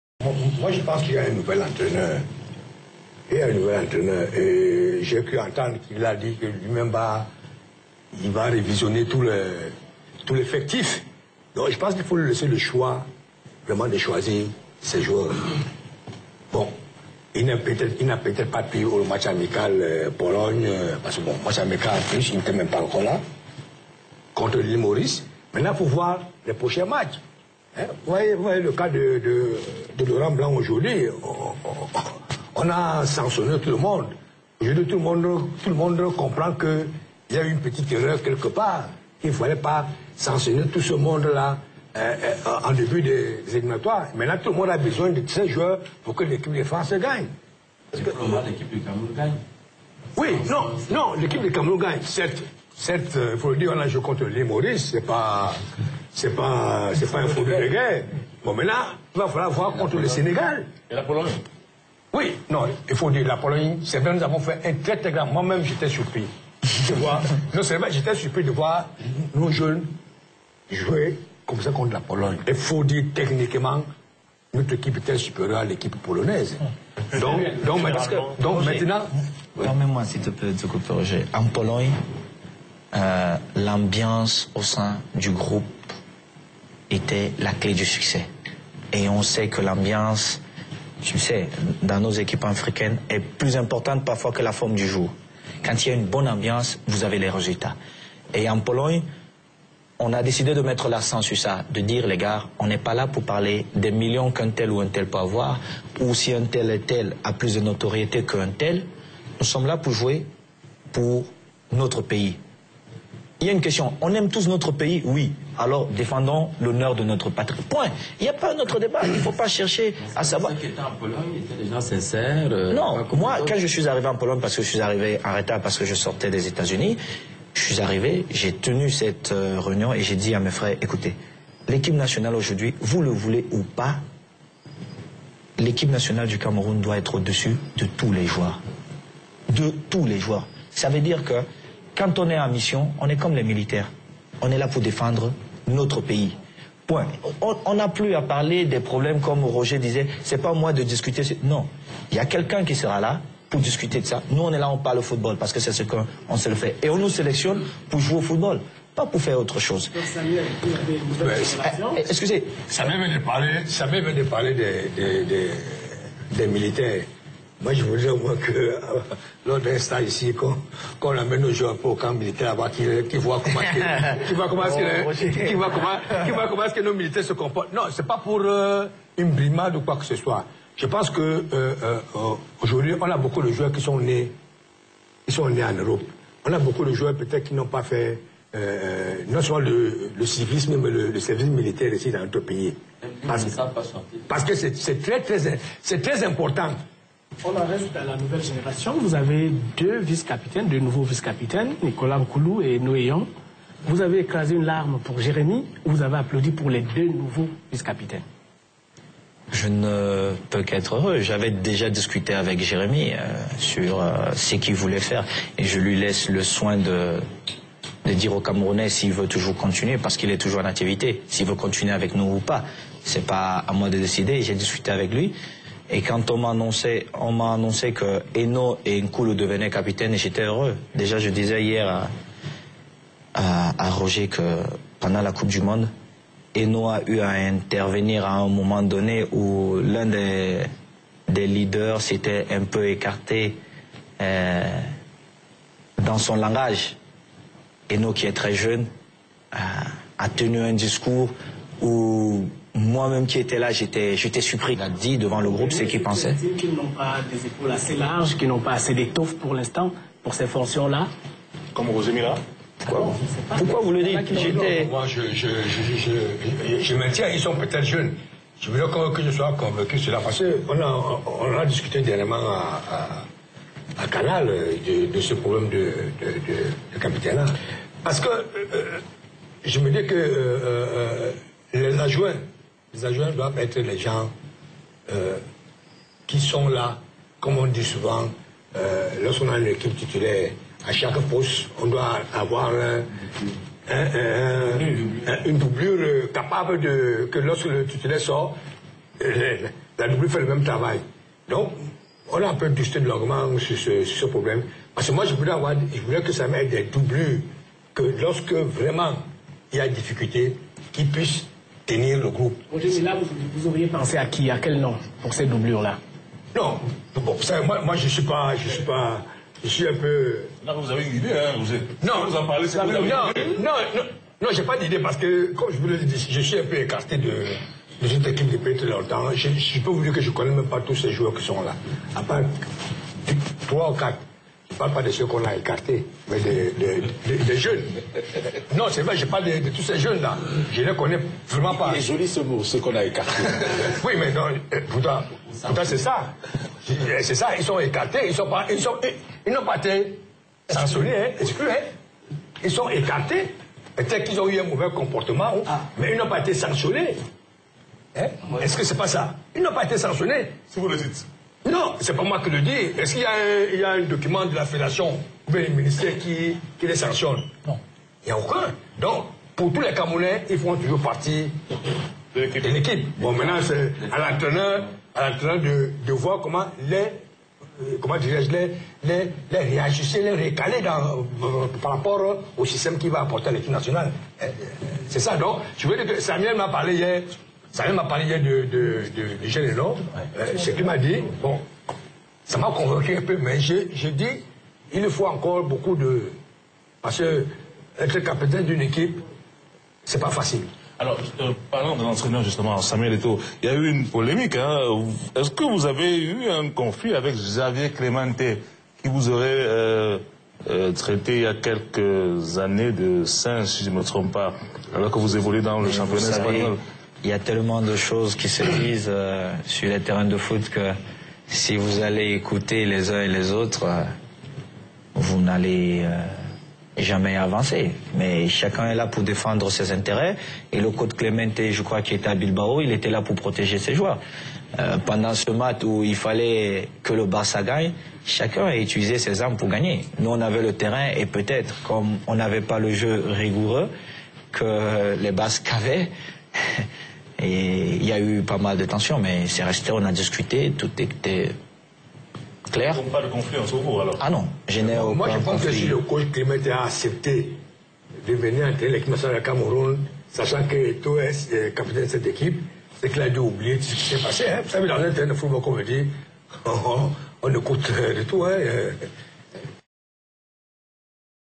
ETO'O se lache prt8: "NKOULOU et ENOH capitaines ,je suis heureux"
Face à la presse jeudi 9 septembre dernier,Eto'o Fils s'est exprimé largement sur le mondial ,les "exclu" ,en présence de Roger Milla .Il n'a pas manqué de rendre un hommage a celui qu'il estime être comme le dieu du football  camerounais ,debat brillammant mené par :
Alain Belibi (CRTV)
Thierry Ngogang (STV)
Jean Materrne Ndi (DBS-TV)
Joly Koum (CANAL 2 inter)
ZINGA (EQUINOXE-TV)